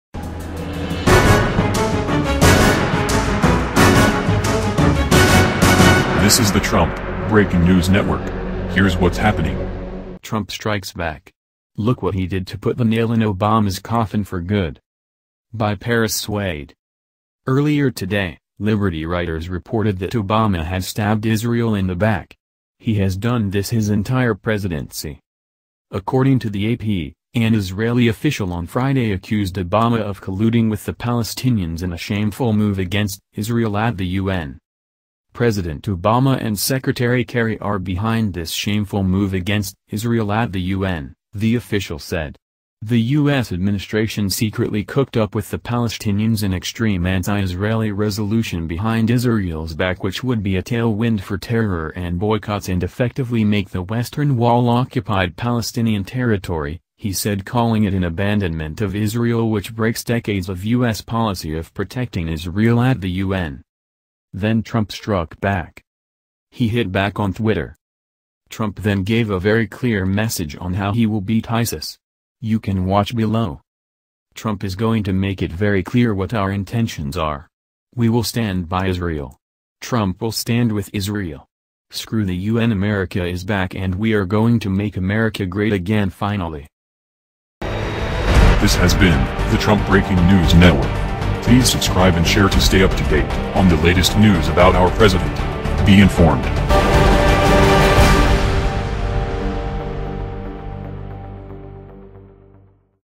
TRUMP STRIKES BACK! Look What He Did To Put The Nail In Obama’s Coffin For Good
Trump Breaking News Network - TRUMP STRIKES BACK! Look What He Did To Put The Nail In Obama’s Coffin For Good.

By Paris Swade.

Earlier today, Liberty Writers reported that Obama had stabbed Israel in the back. He has done this his entire presidency.

According to the AP, an Israeli official on Friday accused Obama of colluding with the Palestinians in a “shameful move against Israel at the U.N.”

“President Obama and Secretary Kerry are behind this shameful move against Israel at the U.N.,” the official said. “The U.S administration secretly cooked up with the Palestinians an extreme anti-Israeli resolution behind Israel’s back which would be a tail wind for terror and boycotts and effectively make the Western Wall occupied Palestinian territory,” he said calling it “an abandonment of Israel which breaks decades of US policy of protecting Israel at the UN.”

THEN TRUMP STRUCK BACK!

He hit back on Twitter.

Trump then gave a very clear message on how he will beat ISIS. You can watch below:

Trump is going to make it very clear what our intentions are. We will stand by Israel! Trump will stand with Israel. Screw the U.N. America is back and we are going to make America great again finally.